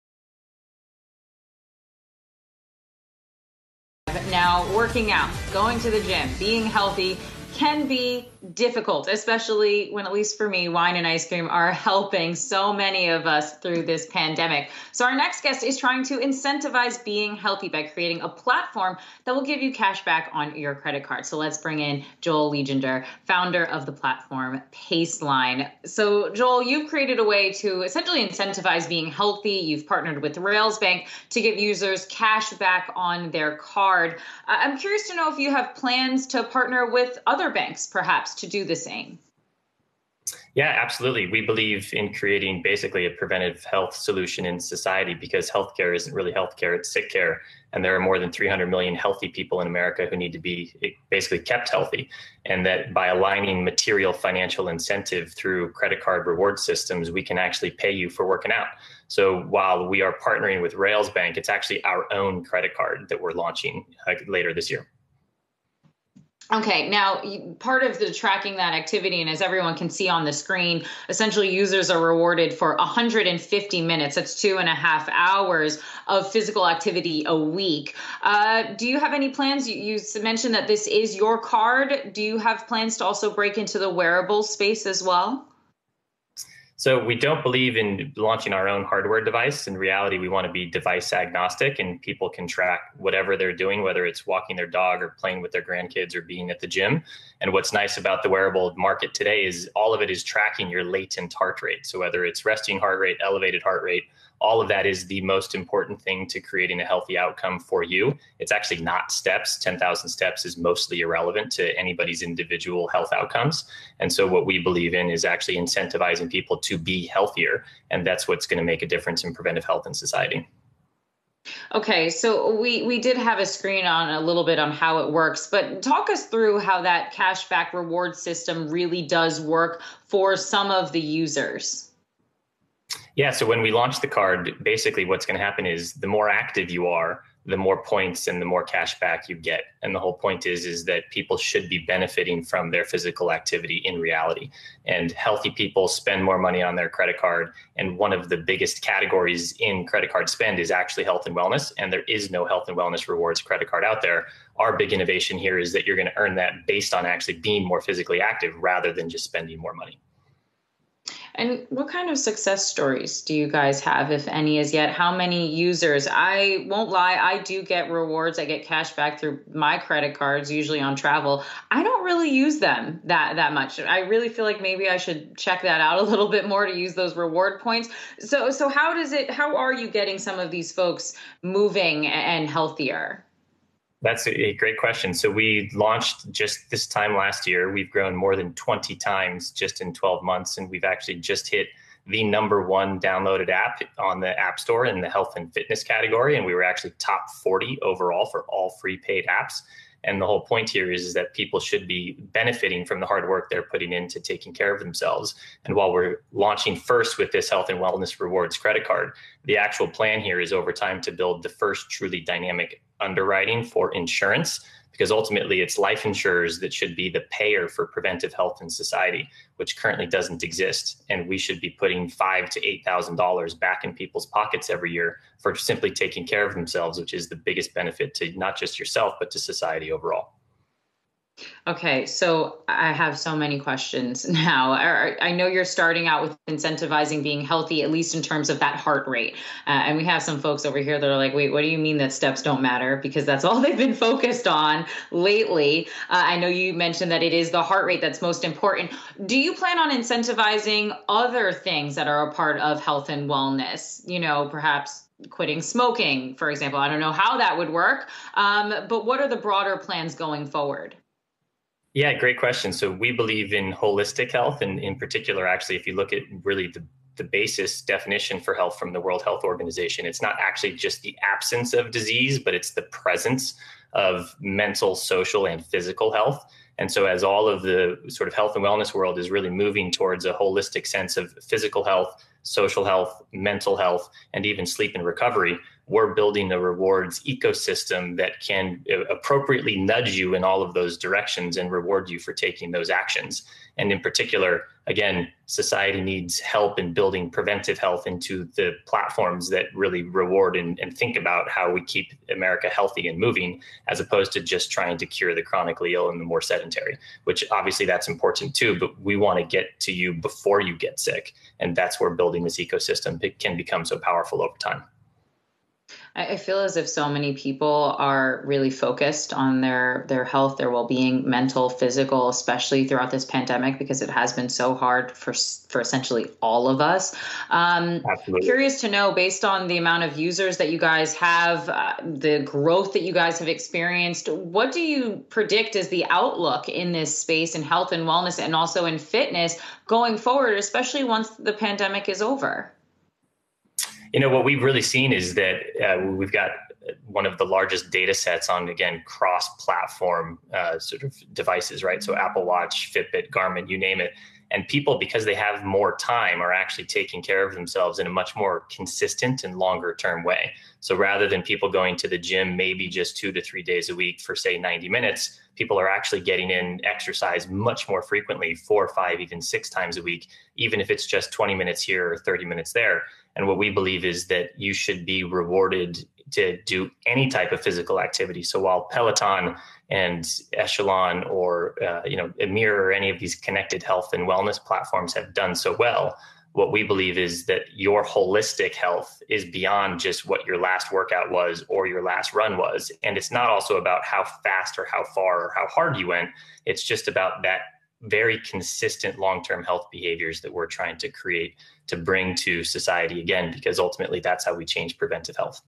Fitness platform pays users to be active and live healthy lifestyles
Yahoo Finance's Kristin Myers spoke to Joel Lieginger Paceline Founder about how his fitness app gives financial rewards to users who stay active and adapt healthy lifestyles.